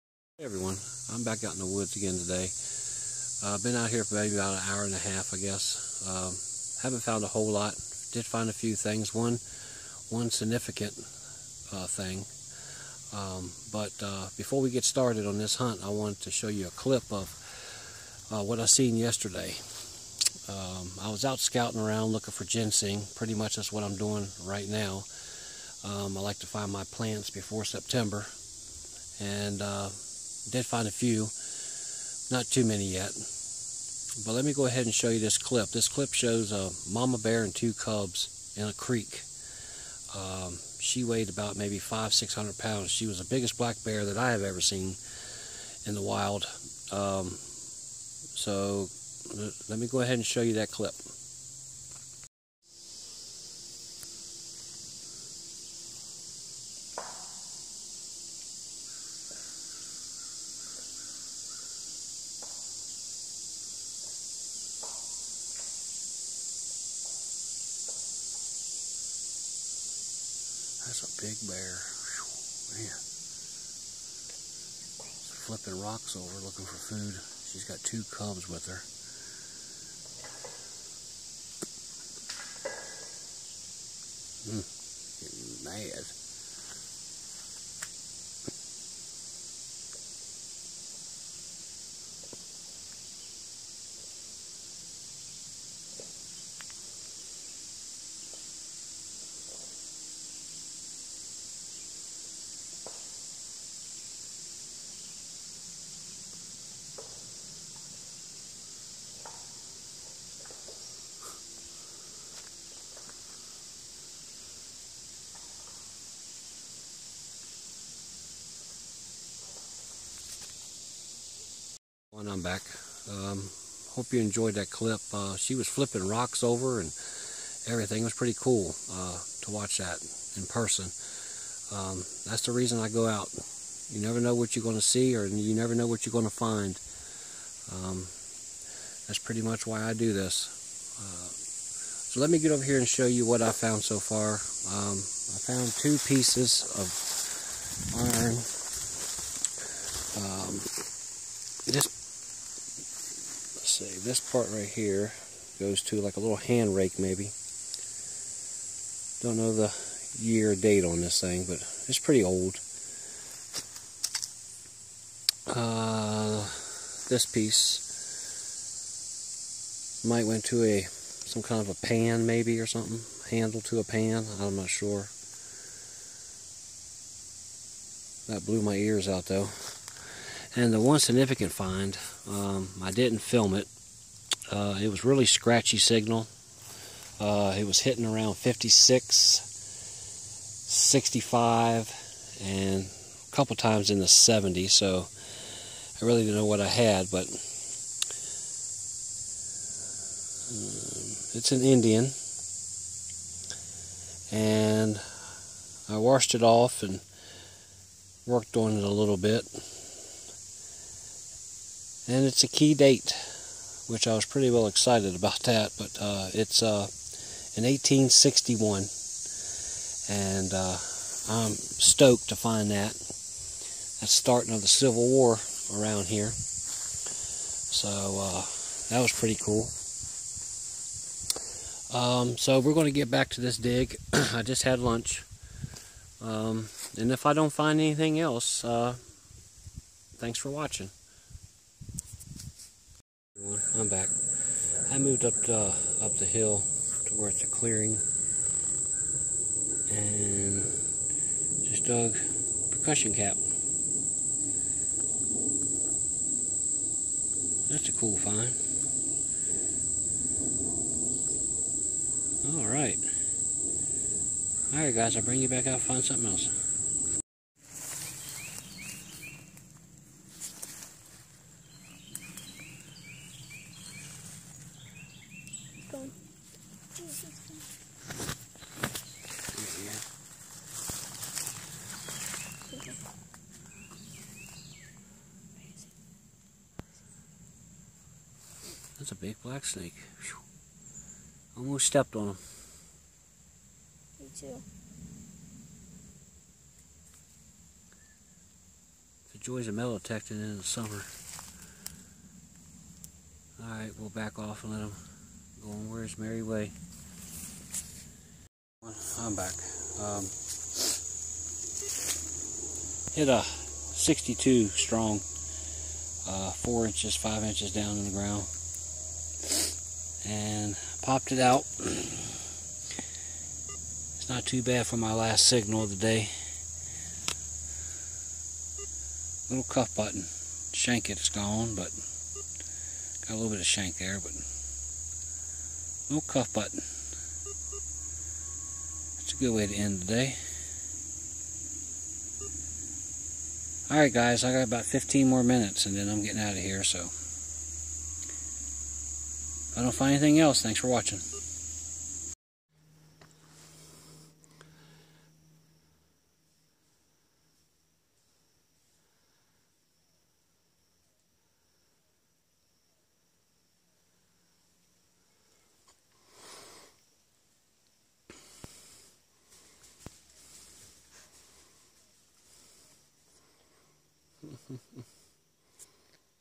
Metal Detecting Key Date IHP Bears Snakes More AT Pro
Join me as i metal detect a new spot in the blue ridge mountains in va